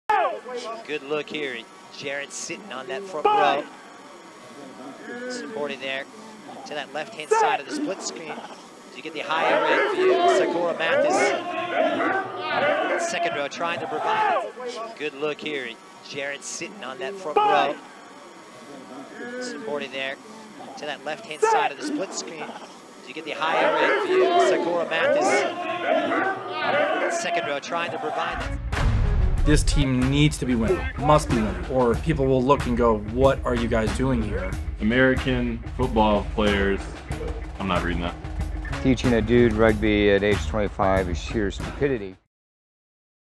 Jeffrey Peleseuma Scrum Hold And Ball Out AMERICAN RAPTORS vs LA RISING STARS 05.28.22
JEFFREY PELESEUMA SCRUM HOLD & BALL OUT